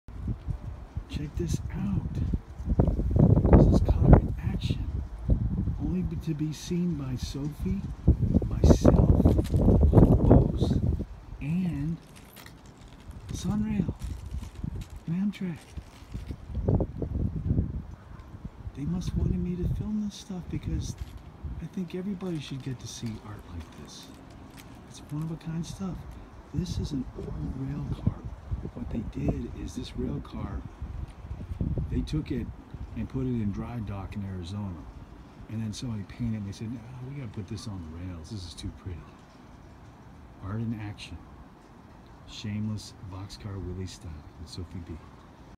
Sophie Be Super Model "Shameless Art In Action Box Car Willie Styli" segment #385
Sophie Be Super Model riding the rails as we spy the "Motherload of Shameless Art Action.